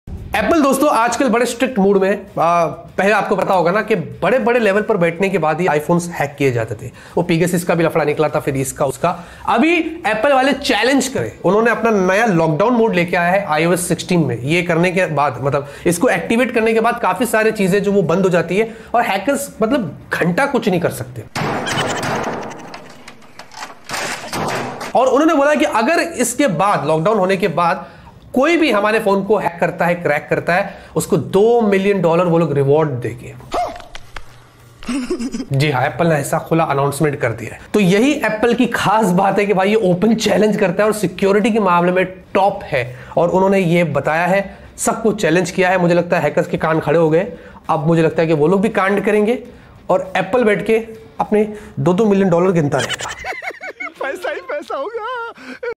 Apple Offering $2 million🤑 to hack/break its New Lockdown Mode | iOS 16 New Update
Iss video me Techno Ruhez aapko bta rhe Apple ke new security feature Lockdown Mode ke baare me jise hack or break karne par Apple aapko $2 million dega.